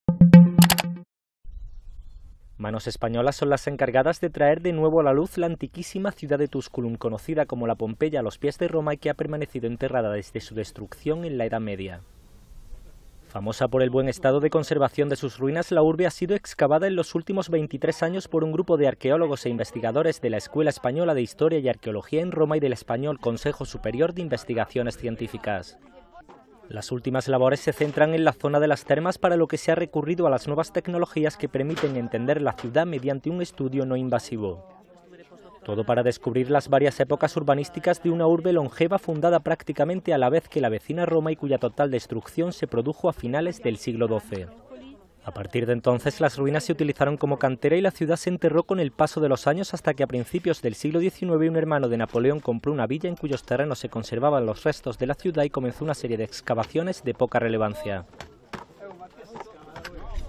Arqueólogos españoles sacan a la luz la ciudad de Tusculum
Roma (Italia), 10 may (EFE).- (Imagen: Jaime Castro) La antiquísima ciudad de Tusculum, conocida como "la Pompeya a los pies de Roma" por el buen estado de conservación de sus ruinas, es desenterrada por un equipo de investigadores españoles, que presentaron hoy sus últimos avances.